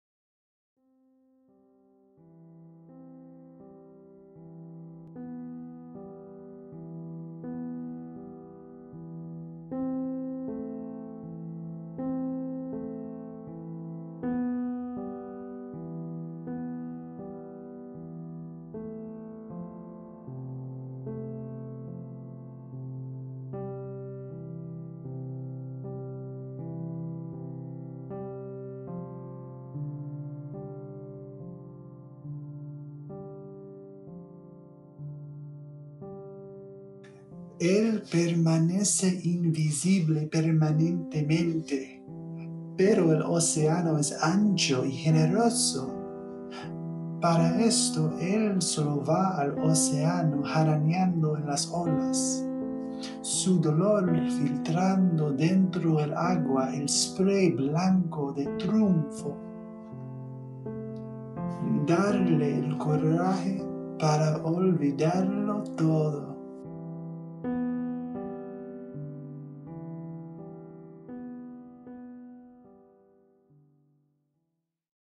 El Surfista Parte Cinco (Un Poema De Fernando Ortiz-Babilonia) (Hecho en Blender 2.83)
Rendido en Blender 2.83 con el motor de render EVEE.
Rendered in Blender 2.83 with the EVEE engine.

Música Usada
Music Used

Gracias Especiales
Special Thanks

Creating Procedural Volumetric Clouds with Blender Eevee by CG Cookie

Muchas gracias a todos mis amigos y amigas que conocí de Tandem por su ayuda con mi español. No pude hacer esta proyecto sin tu ayuda. Todo ustedes son impresionante!

Thank you very much to my friends I met from Tandem for your help with my Spanish. I could not do this project without your help. You all are awesome!